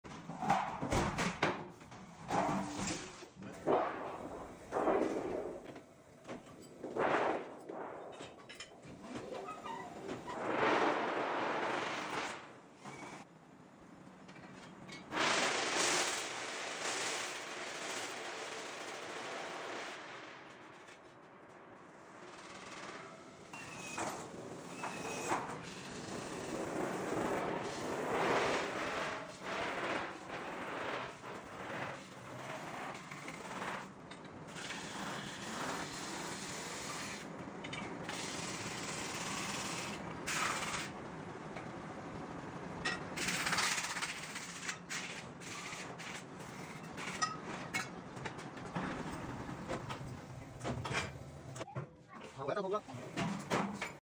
Metal Magic! Building a Satellite Dish from Scratch
Ever wondered how those satellite dishes get made? Watch a metalworking master transform raw stainless steel into a high-performance dish antenna body!  This short video takes you through the incredible process, from cutting and shaping to the final assembly.  Get a glimpse into the skills needed for this fascinating craft!